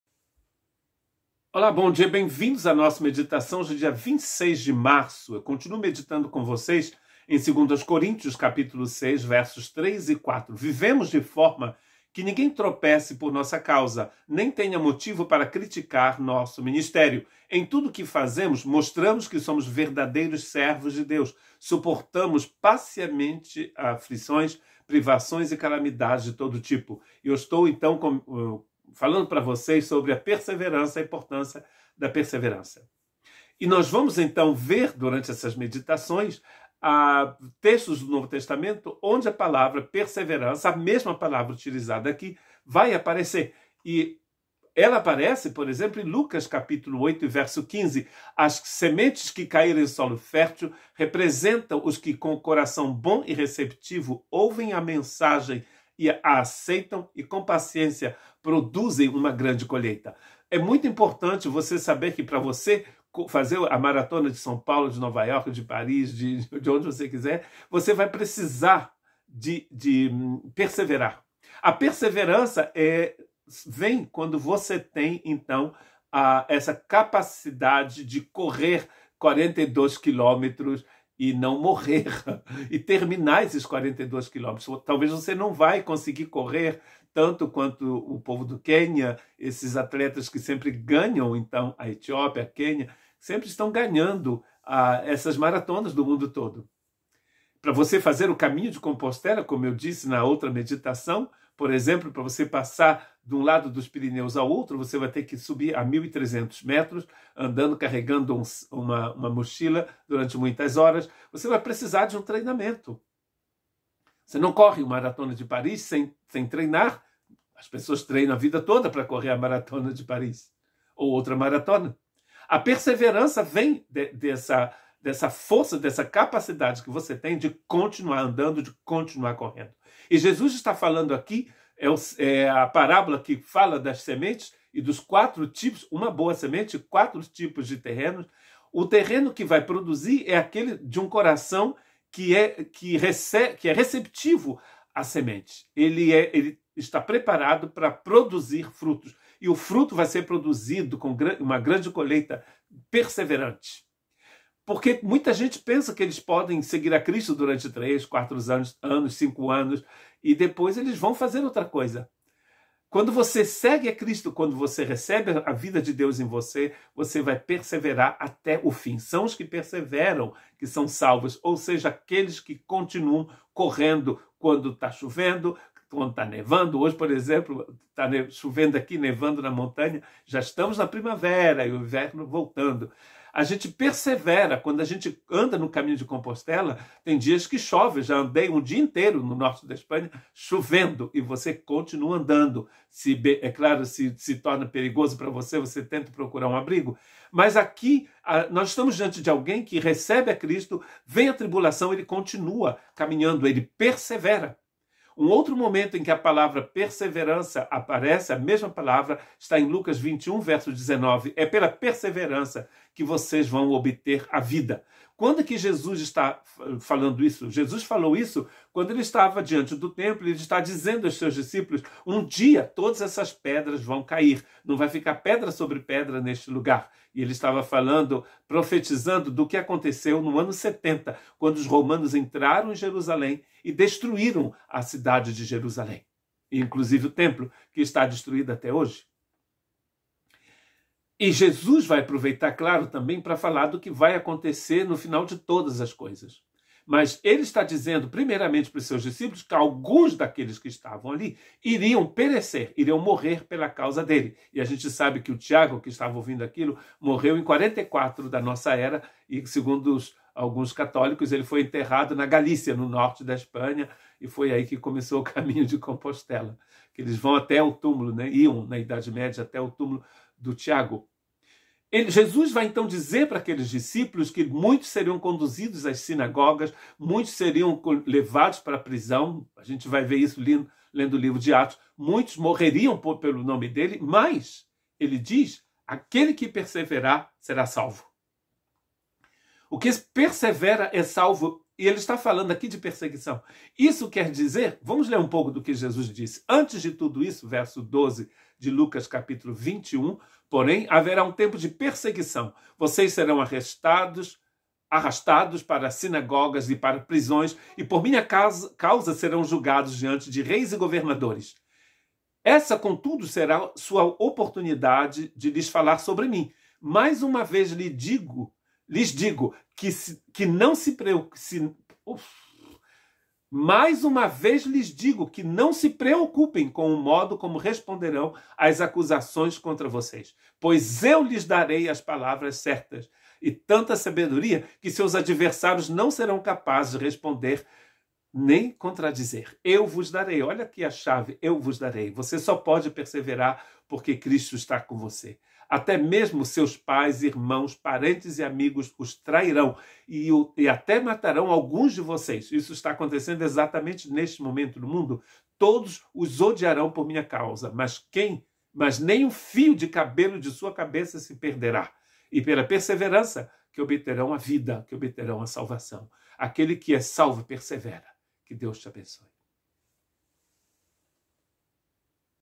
🇧🇷 26 de março 2024. É perseverando que vocês obterão a vida. Junior Damaceno
Mas as que caíram em boa terra são os que, com coração bom e generoso, ouvem a palavra, a retêm e dão fruto, com perseverança.
Lucas 8:15